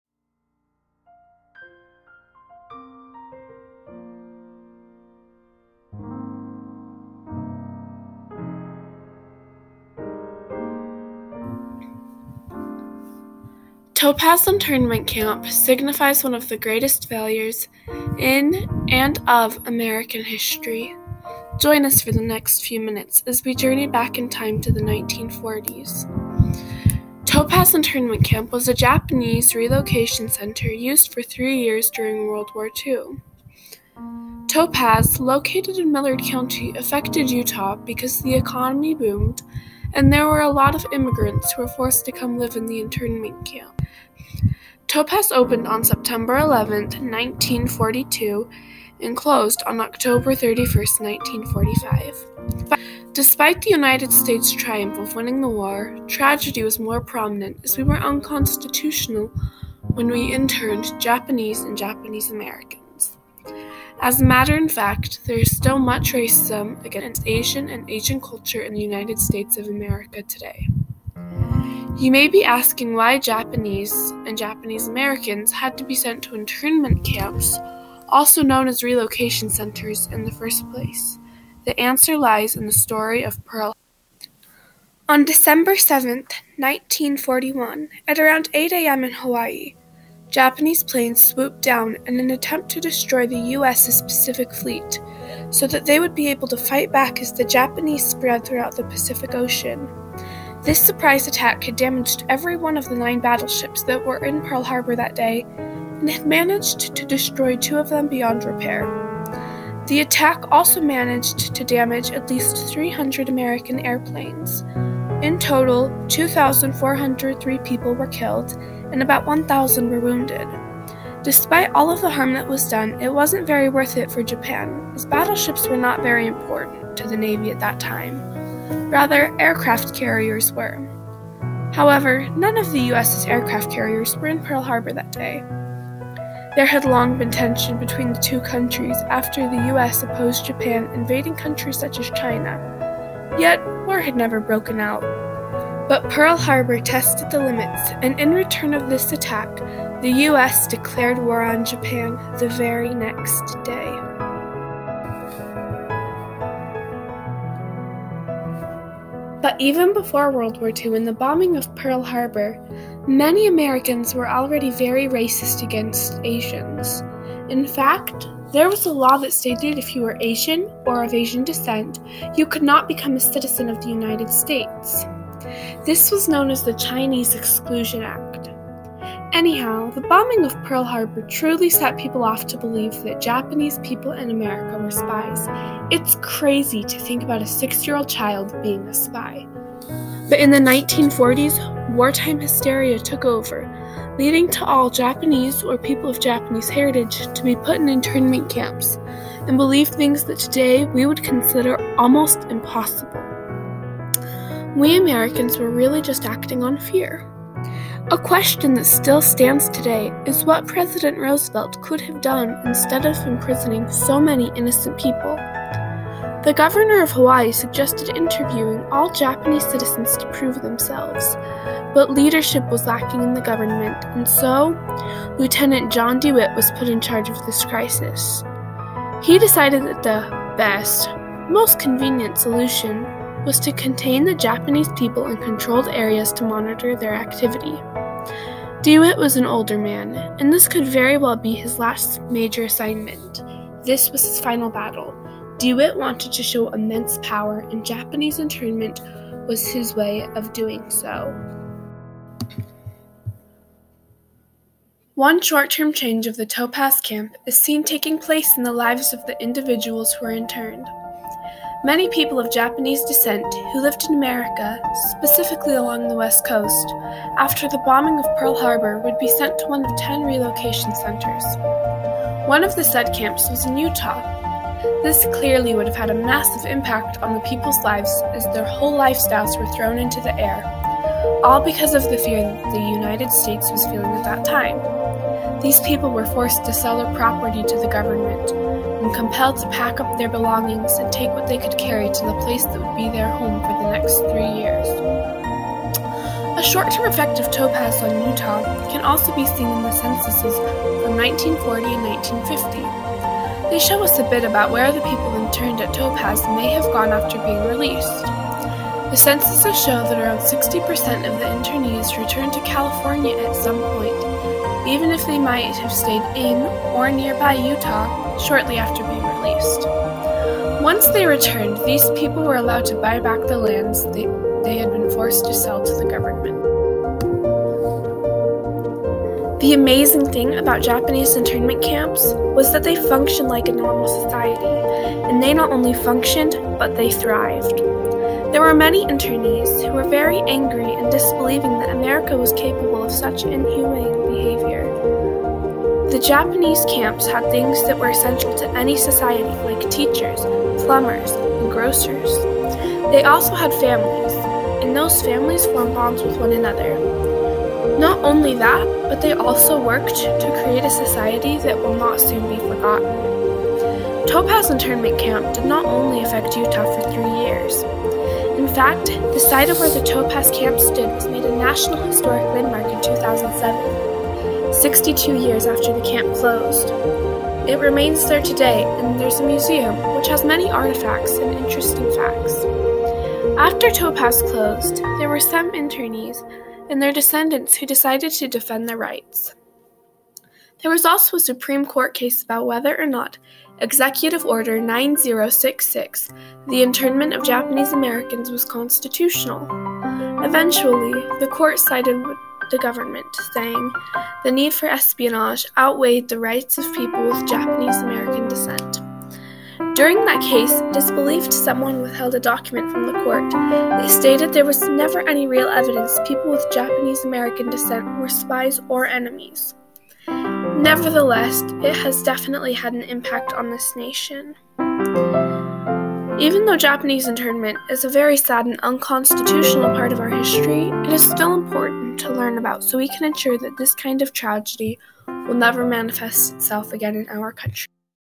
Topaz Internment Camp--Its Context, History, and Why It Was so Unconstitutional
Junior Group Documentary, by Elline Harrison, Ashlee Jensen, and Marissa Parker